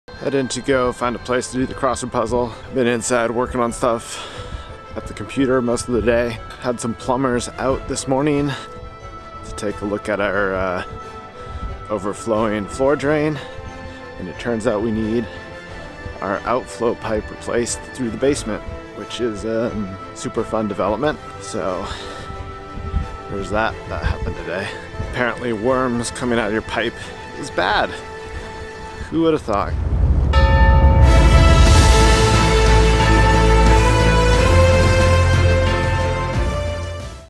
G Log 574: Just the crossword |GH5s vlog
Just walking down to Saraveza to work on the crossword puzzle away from the house.